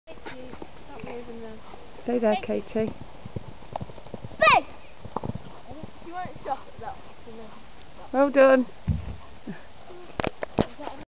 Me and Bramble 2
This is me and bramble 2 jumping a smaller jump